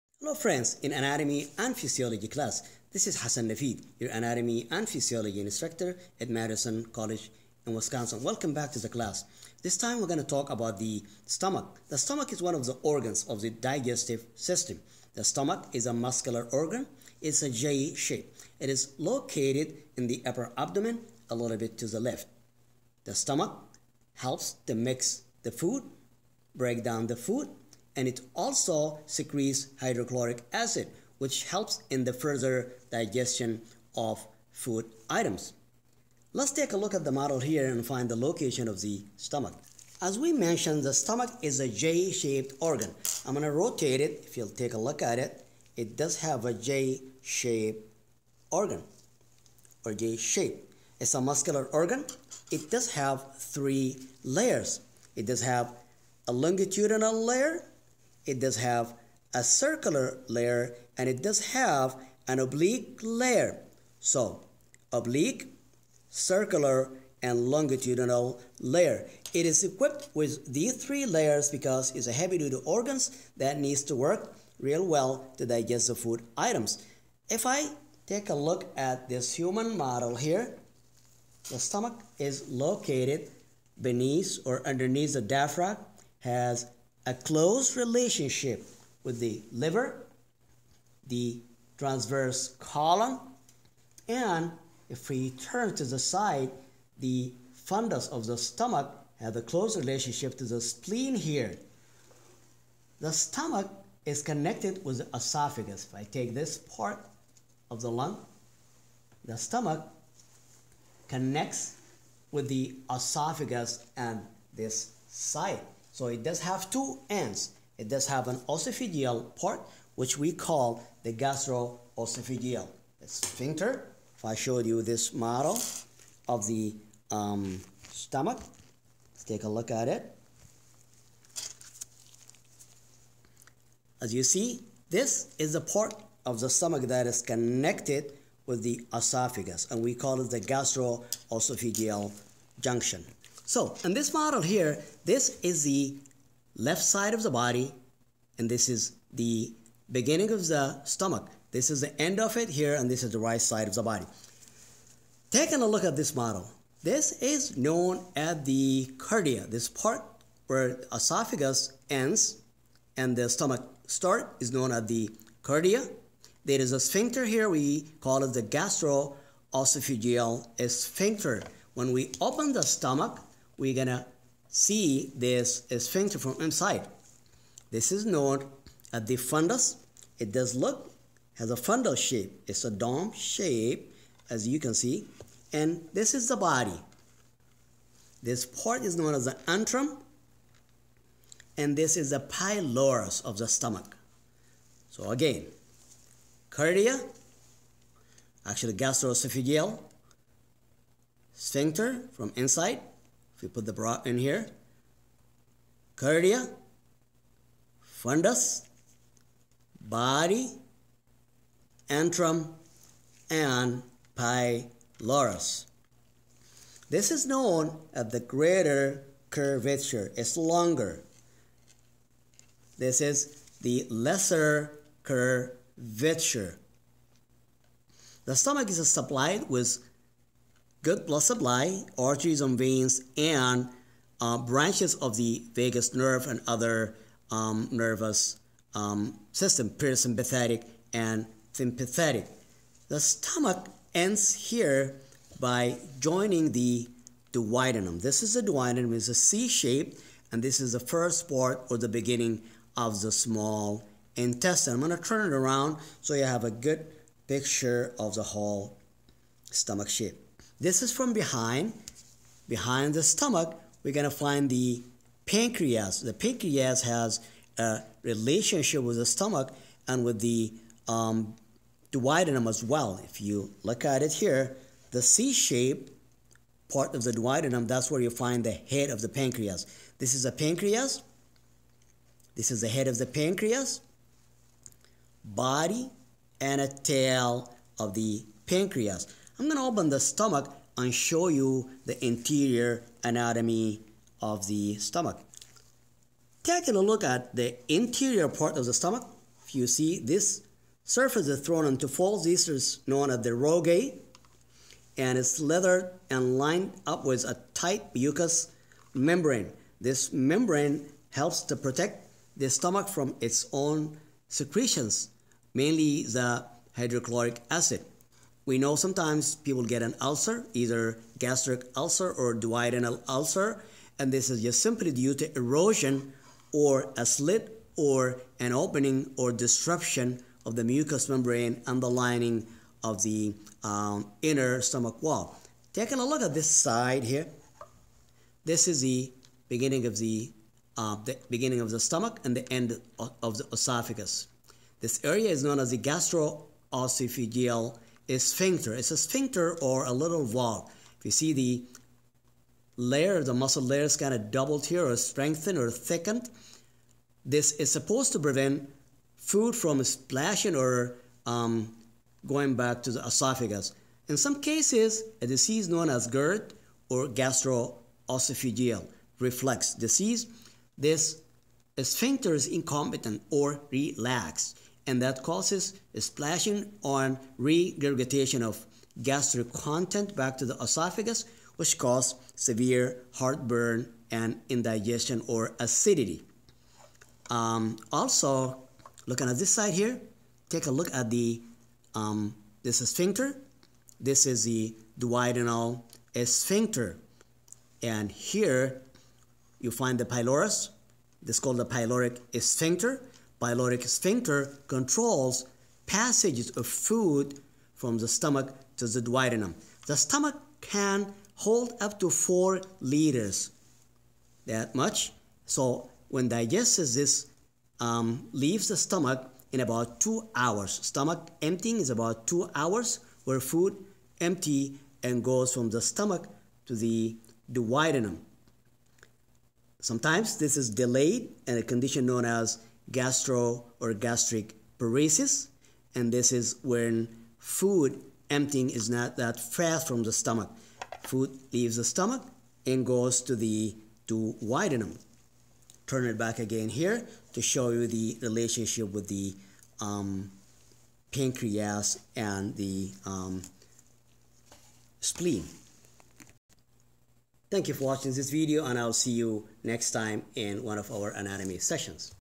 Anatomy of the stomach
Anatomy and relationship of the stomach. Models guided